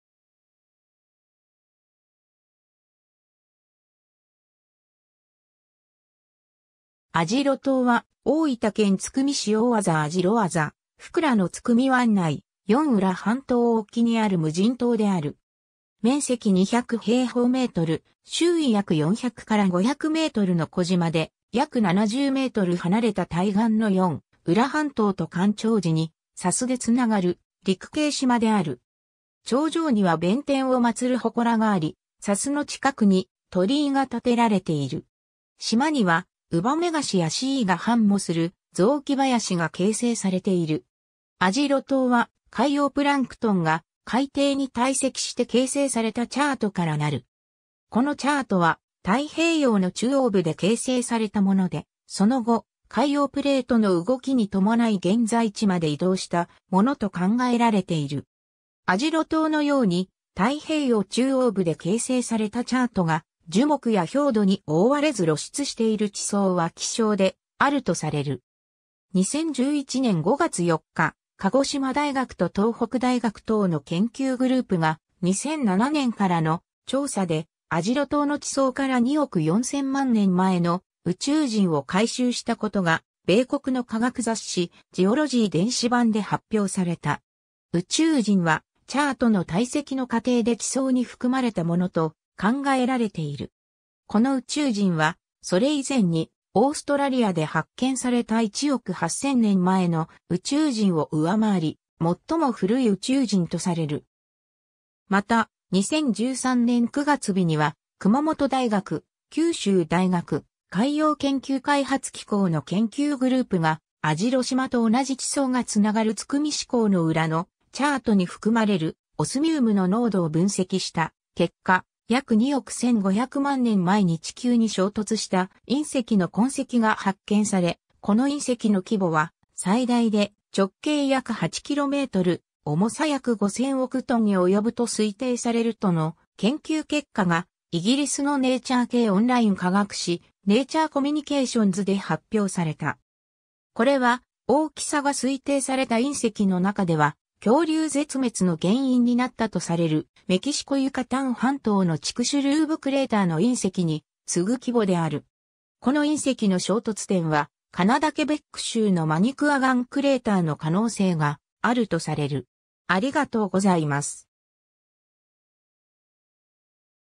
網代島
網代島（あじろじま）は、大分県津久見市大字網代字福良の津久見湾内、四浦半島沖にある無人島である。
 面積200m2、周囲約400-500mの小島で、約70m離れた対岸の四浦半島と干潮時に砂州で繋がる陸繋島である。
 頂上には弁天を祀る祠があり、砂州の近くに鳥居が建てられている。
島にはウバメガシやシイが繁茂する雑木林が形成されている。
 網代島は、海洋プランクトンが海底に堆積して形成されたチャートから成る。
このチャートは太平洋の中央部で形成されたもので、その後、海洋プレートの動きに伴い現在地まで移動したものと考えられている。
網代島のように、太平洋中央部で形成されたチャートが、樹木や表土に覆われず露出している地層は希少であるとされる。
 2011年5月4日、鹿児島大学と東北大学等の研究グループが、2007年からの調査で網代島の地層から2億4千万年前の宇宙塵を回収したことが、米国の科学雑誌『ジオロジー』電子版で発表された。
宇宙塵はチャートの堆積の過程で地層に含まれたものと考えられている。
この宇宙塵は、それ以前にオーストラリアで発見された1億8千年前の宇宙塵を上回り、最も古い宇宙塵とされる。
 また、2013年9月日には、熊本大学、九州大学、海洋研究開発機構の研究グループが、網代島と同じ地層がつながる津久見市江ノ浦のチャートに含まれるオスミウムの濃度を分析した結果、約2億1500万年前に地球に衝突した隕石の痕跡が発見され、この隕石の規模は最大で直径約8km、重さ約5千億tに及ぶと推定されるとの研究結果が、イギリスのネイチャー系オンライン科学誌『ネイチャー コミュニケーションズ』で発表された。
これは、大きさが推定された隕石の中では、恐竜絶滅（K-Pg境界）の原因になったとされるメキシコ・ユカタン半島のチクシュルーブ・クレーターの隕石に次ぐ規模である。
この隕石の衝突点は、カナダ・ケベック州のマニクアガン・クレーターの可能性があるとされる。